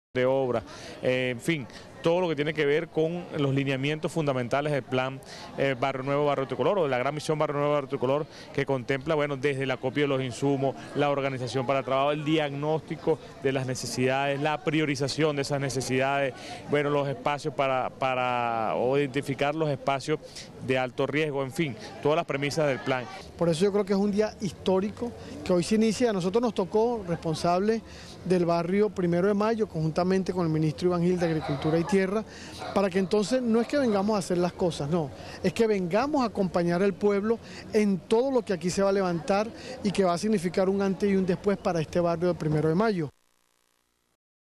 Ministros Yván Gil y Miguel Rodríguez - Gran Misión Barrio Nuevo Barrio Tricolor
Ministros revolucionarios desde el Barrio 1° de Mayo, El Cementerio - 1er Eje Corredor Valle-Coche del área metropolitana de Caracas